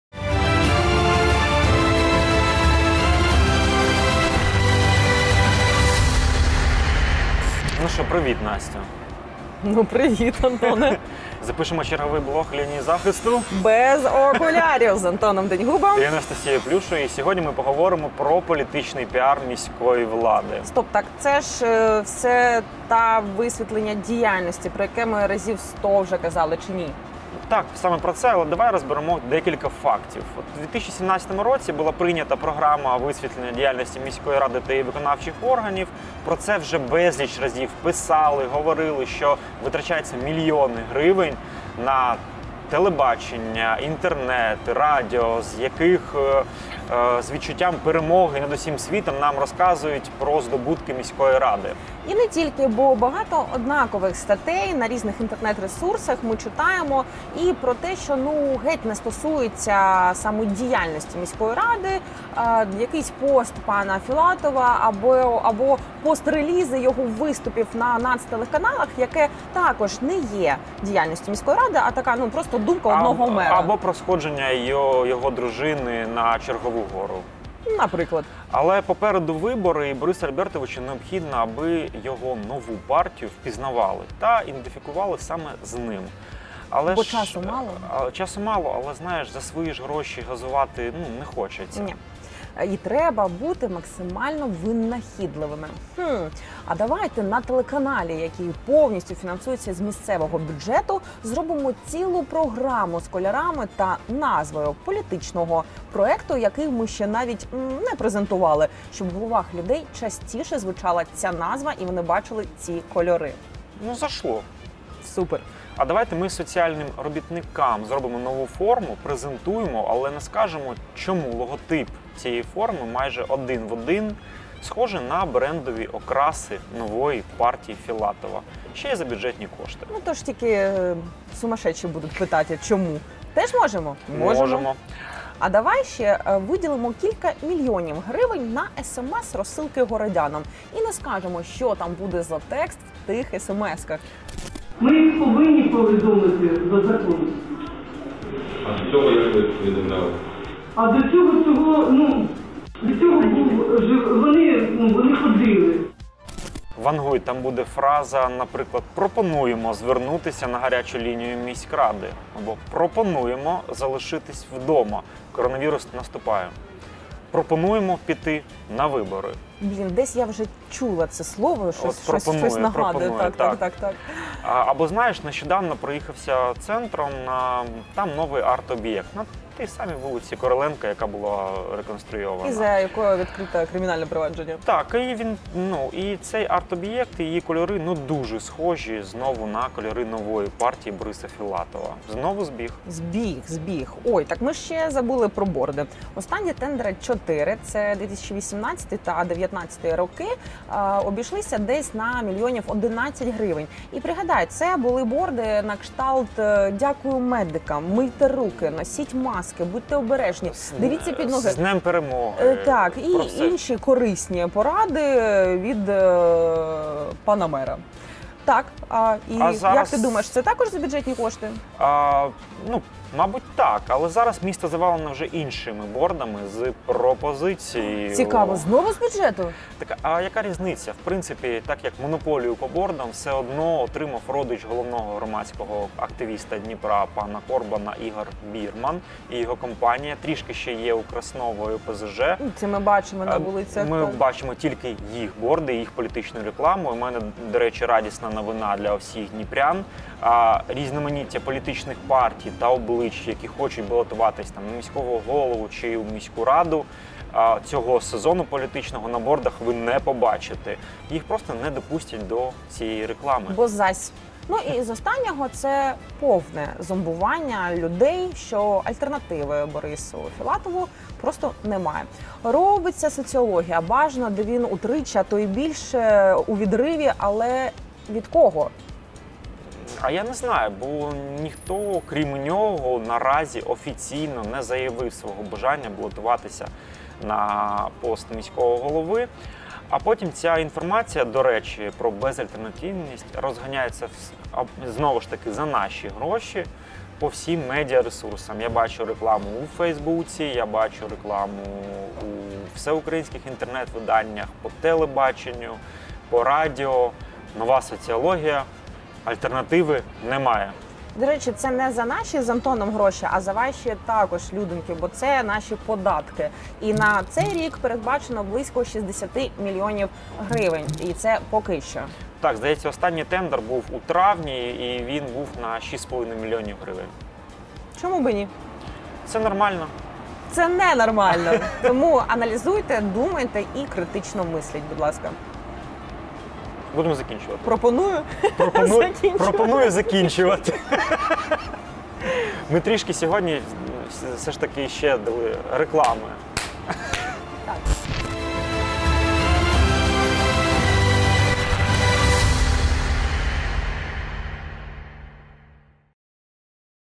ПРО....
ПРОдукти..ПРОмо..ПРОєкт...ПРОфіль ... і про інше "ПРО" 🤔

ПРО те, як за наші з вами гроші піаряться ті, кому ми дозволу не давали. І саме вони лишають нас як мінмум візуального вибору 💁‍♀️

Новий блог від ЛініяЗахисту